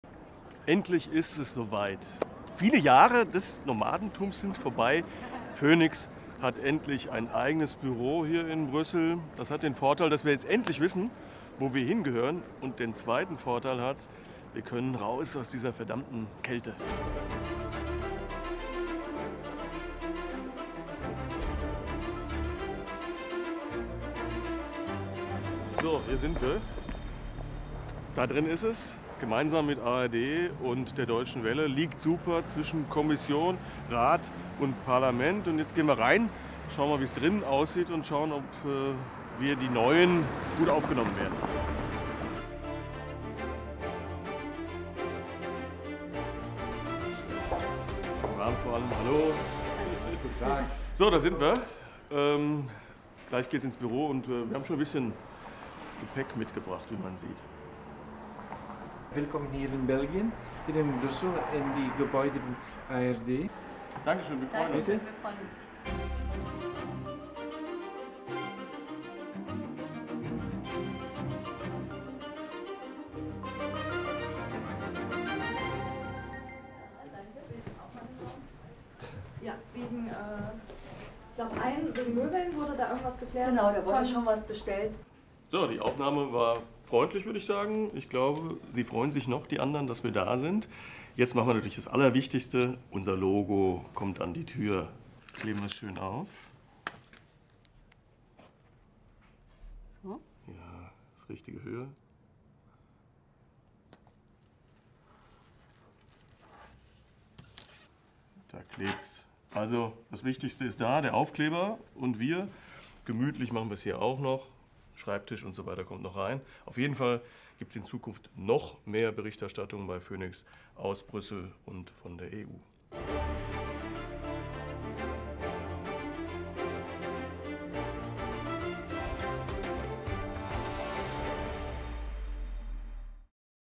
Besichtigungstermin - phoenix baut seine Europa-Berichterstattung aus
phoenix baut seine Europa-Berichterstattung weiter aus und bezieht in Kürze ein eigenes kleines Bürozimmer in Brüssel. Unser EU-Reporter Klaus Weber und sein Team nehmen Sie mit zum 2Besichtigungstermin".